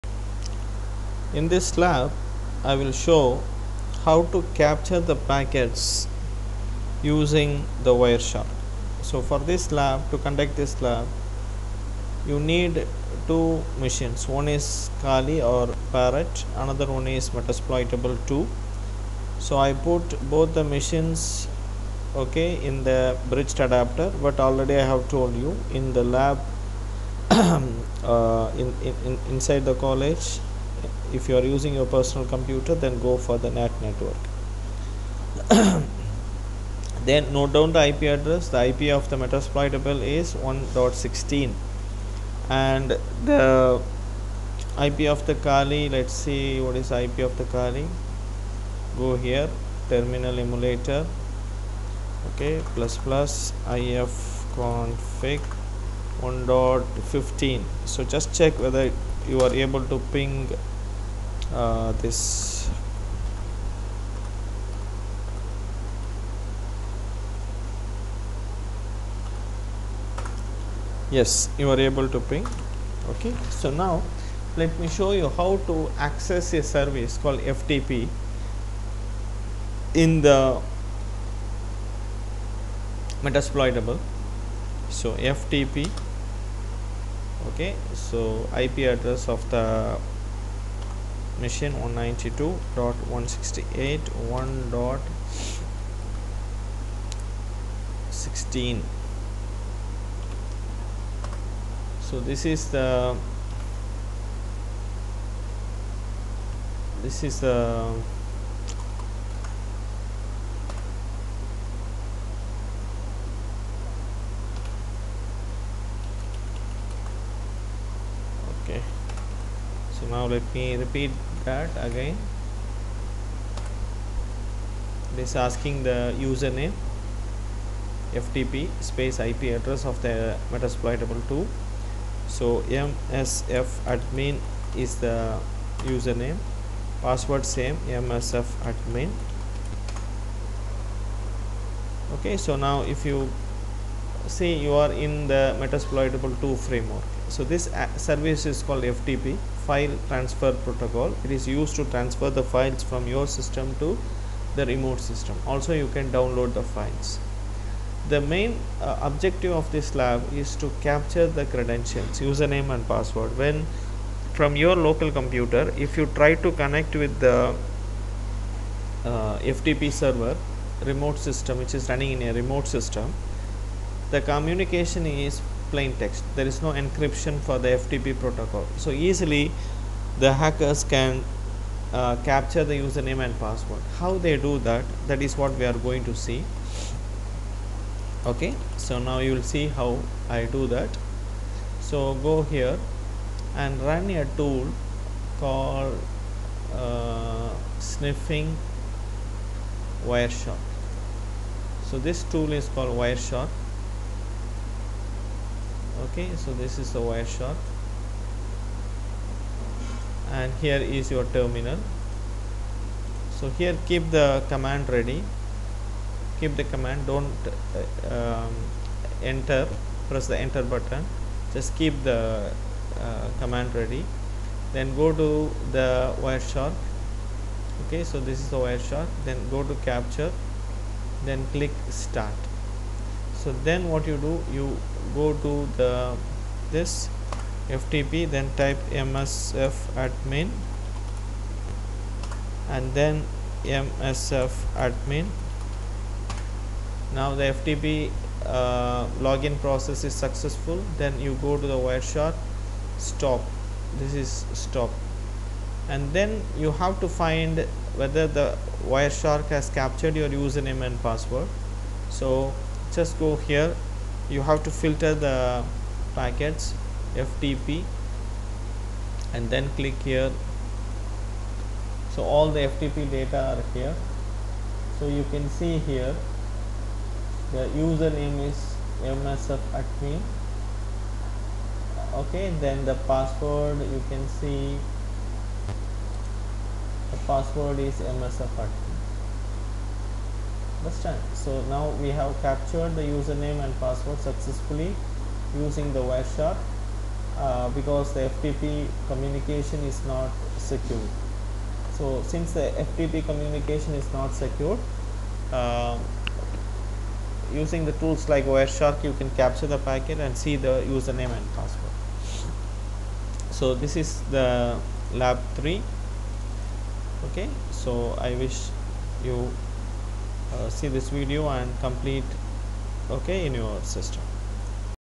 FTP Credentials Sniffing
Set UP Kali
Set up Metasploitable 2
configure Network Adapter [ Bridged or NAT Network]
Run Wireshark
Connect FTP from KALI with Metasploitable 2 IP @
Capture the Packet and search the credential's